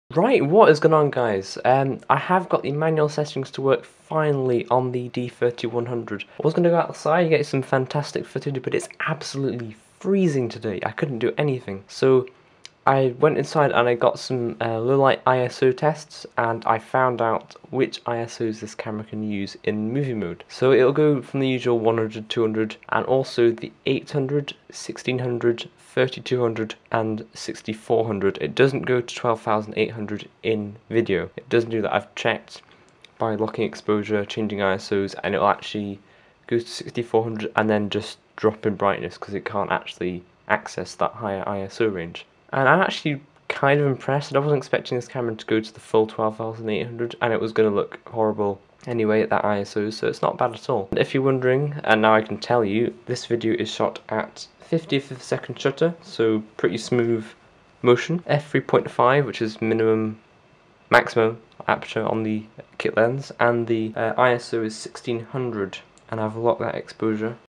Nikon D3100 Manual Video ISO Tests
A test of ISO performance of Nikon D3100 Shooting full manual video.

ISO range from 100 to 6400.